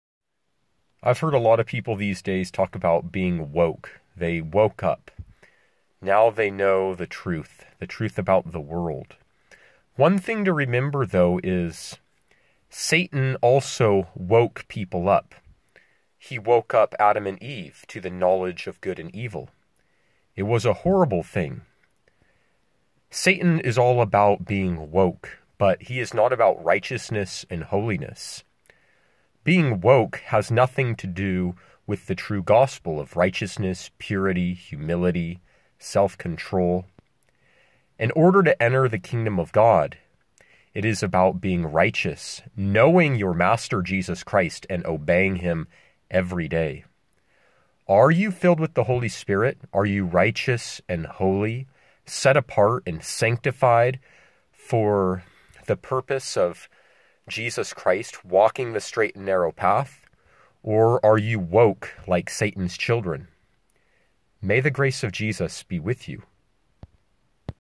Satan's Children Are "Woke"
I’ve heard a lot of people these days talking about being “WOKE”, they woke up, now they know the truth… the truth about the world.  One thing to remember though is, satan also woke people up, he woke up Adam and Eve to the knowledge of good and evil.  It was a horrible thing.  Satan is all about being woke, but he is not about righteousness and holiness.  Being “woke”  has nothing to do with the true gospel of righteousness, purity, humility, self-control… in order to enter the Kingdom of God it  is about being RIGHTEOUS, knowing your Master Jesus Christ and obeying Him every day.  Are you filled with the Holy Spirit?  Are you righteous and holy, set apart and sanctified for the purpose of Jesus Christ, walking the straight and narrow path?  Or are you “woke” like satan’s children?  May the grace of Jesus be with you.